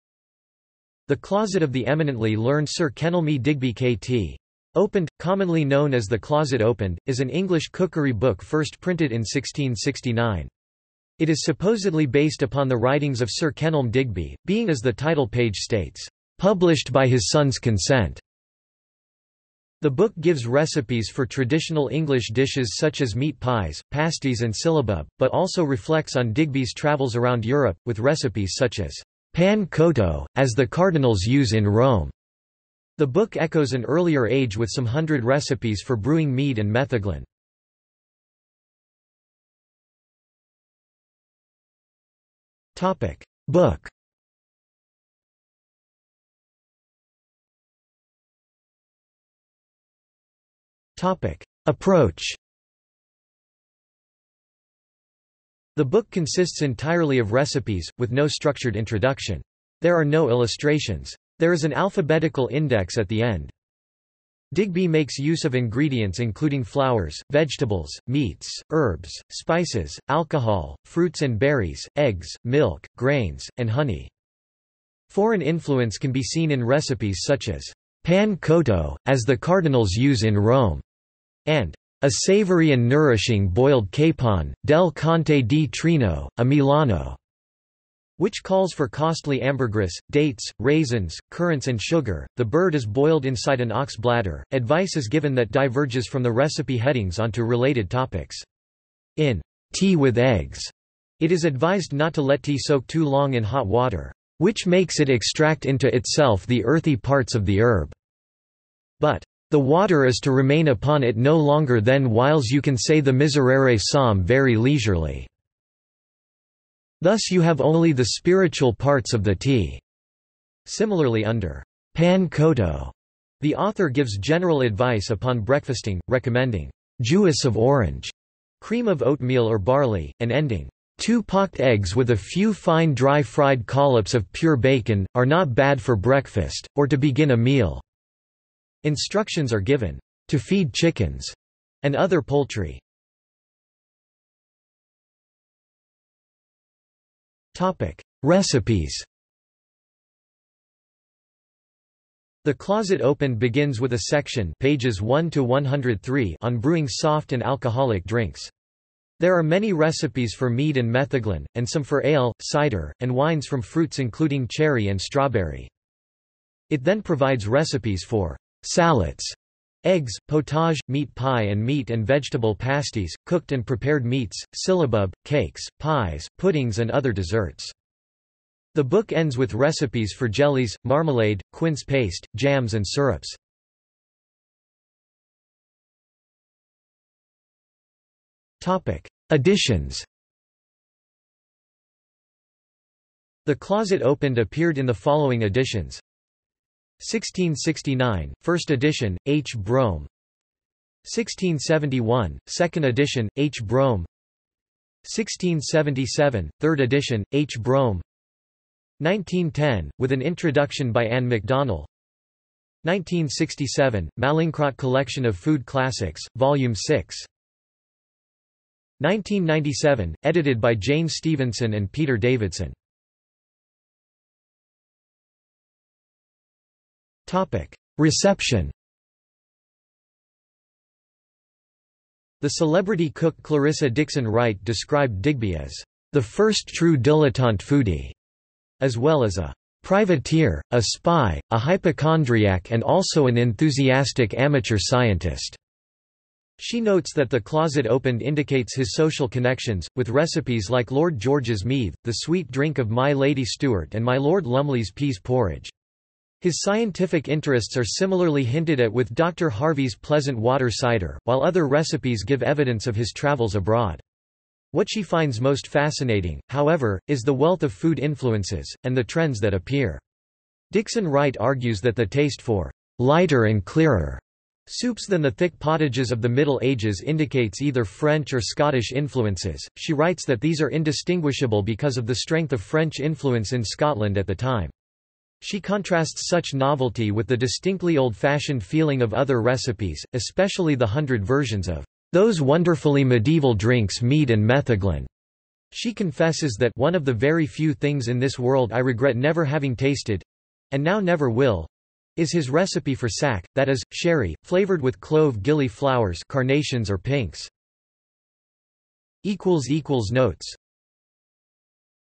The Closet of the Eminently Learned Sir Kenelme Digbie Kt. Opened | Wikipedia audio article
This is an audio version of the Wikipedia Article:
The Closet of the Eminently Learned Sir Kenelme Digbie Kt. Opened

00:00:39 1 Book
00:00:47 1.1 Approach
00:02:24 1.2 Recipes
00:03:05 1.3 Editions
00:03:43 2 Reception

- Socrates

SUMMARY
The Closet of the Eminently Learned Sir Kenelme Digbie Kt. Opened, commonly known as The Closet Opened, is an English cookery book first printed in 1669. It is supposedly based upon the writings of Sir Kenelm Digby, being as the title page states "published by his son's consent".
The book gives recipes for traditional English dishes such as meat pies, pasties and syllabub, but also reflects on Digby's travels around Europe, with recipes such as "Pan Cotto, as the Cardinals use in Rome". The book echoes an earlier age with some hundred recipes for brewing mead and metheglin.